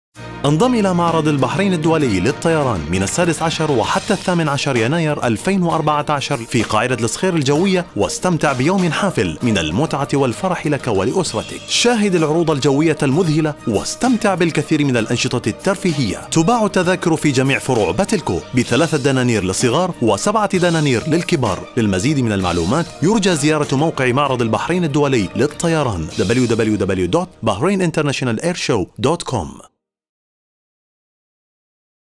Bahrain International Air Show 2014 TV Commercial 1- Arabic Version
Bahrain International Air Show TV Commercial 2014 for the Public Event - The Arabic Version
Produced by : ad.mark
Client : Bahrain International Air Show
Agency : GulfMarcom